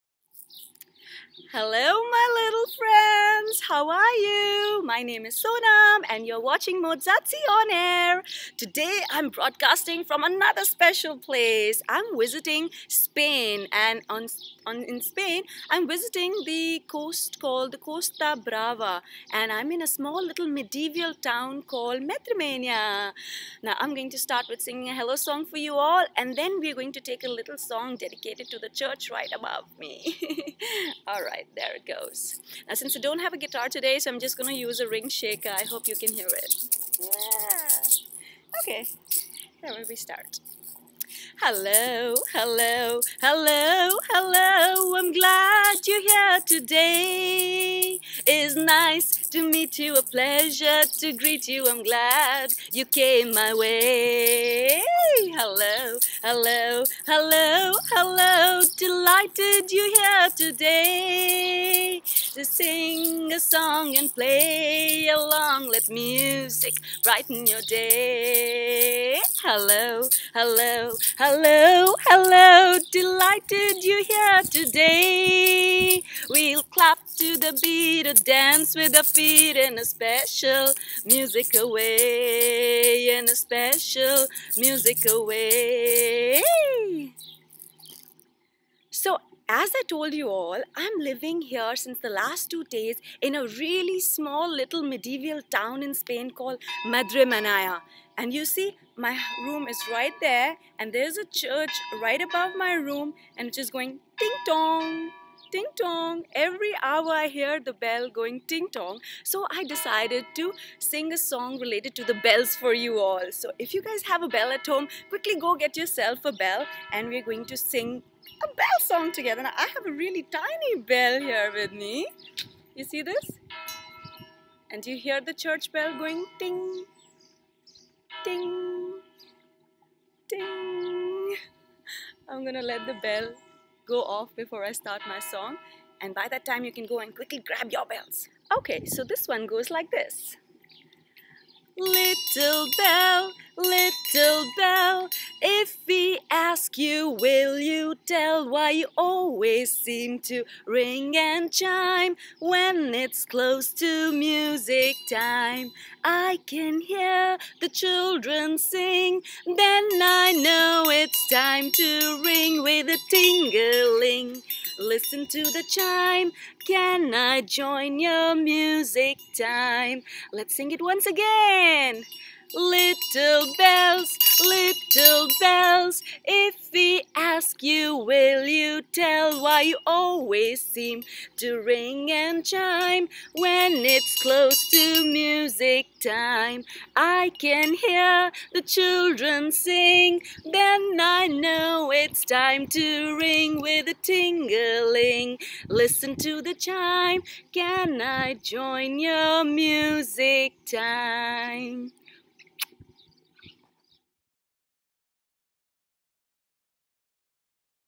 Mozartsy On Air - Little Bells from Costa Brava, SPAIN :)
Hangout with Sonam Aunty who is broadcasting this time in front of a castle like church in a small medieval town in Costa Brava, SPAIN. Time to sing along with the church bells ringing :)

Click here and join in with your child for this week's musical class at the comfort of your home!
Like this video and drop us a comment :)

Let the Musical Fun Continue :)